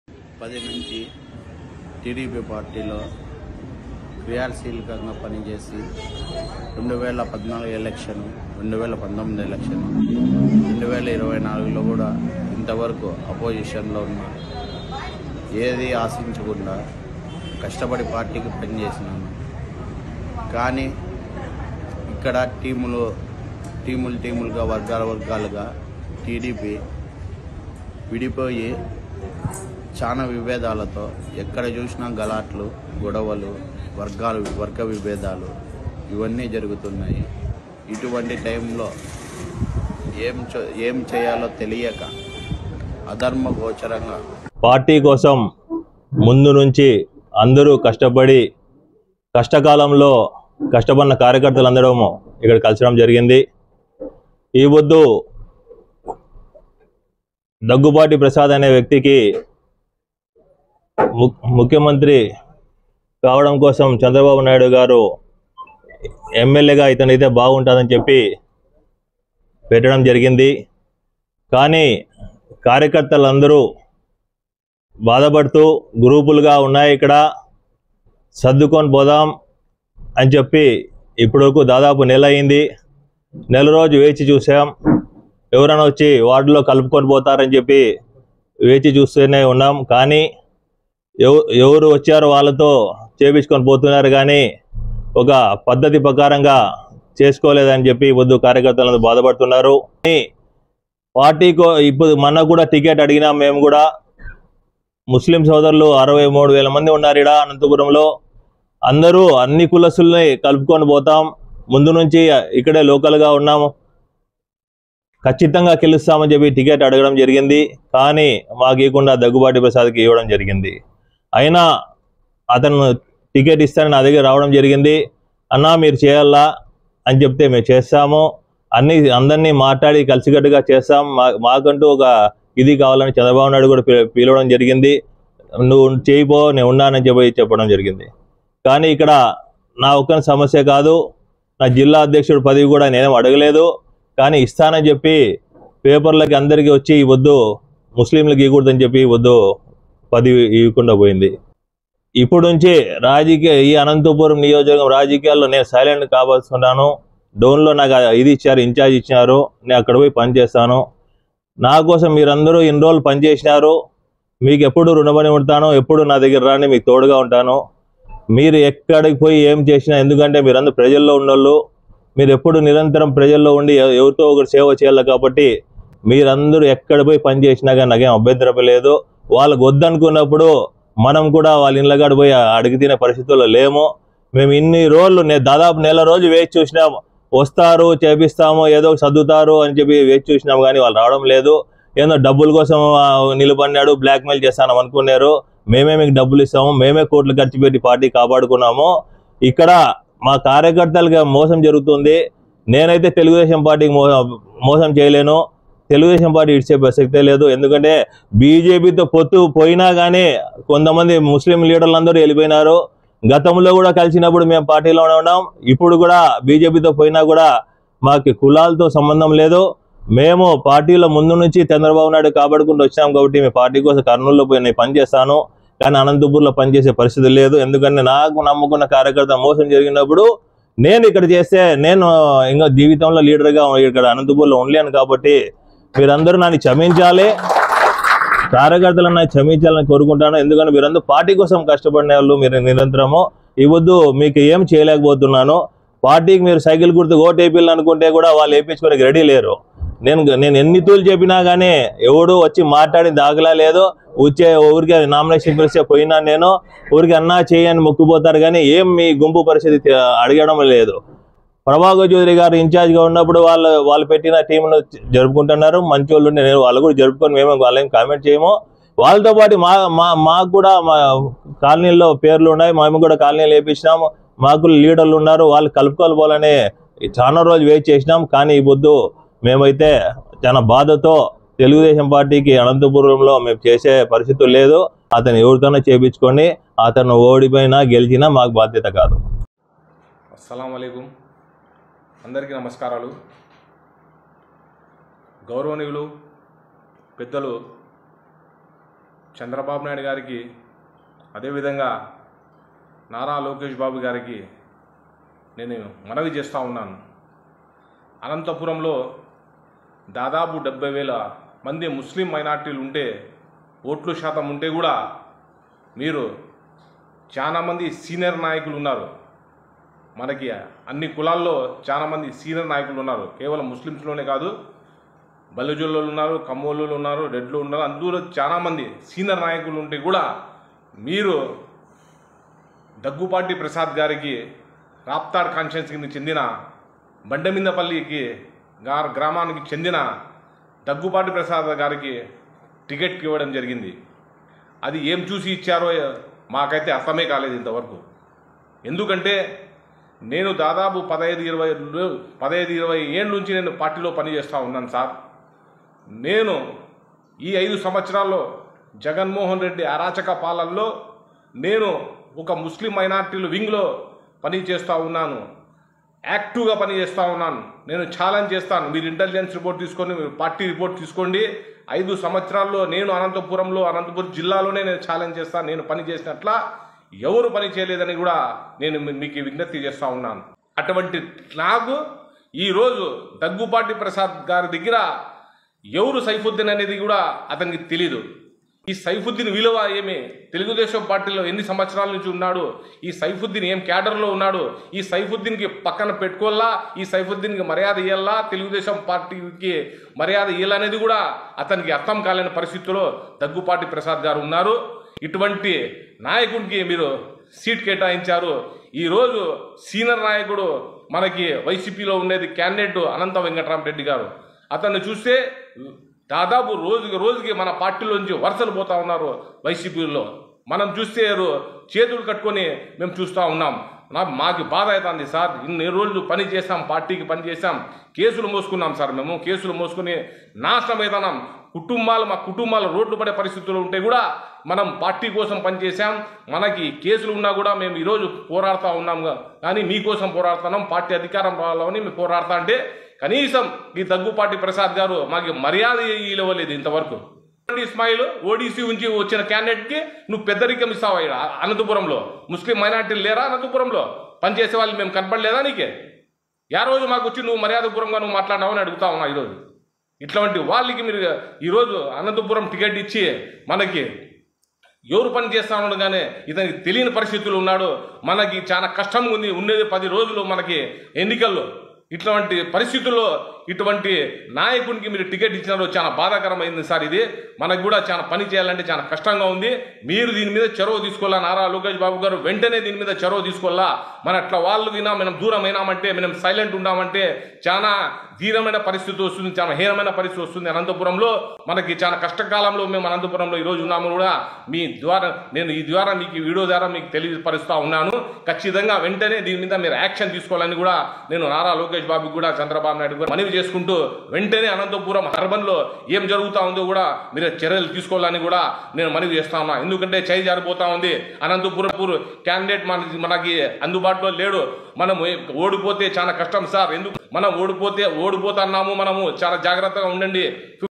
దయనీయ పరిస్థితిలో అనంతపురం టీడీపీ.. ఎన్నికలకు ముందే చేతులెత్తేస్తున్న నాయకులు.! // tdp // anantapur
మమ్మల్ని ఎవ్వరూ పట్టించుకోలేదు.. మాకు దిక్కెవ్వరు బాబు గారు.!?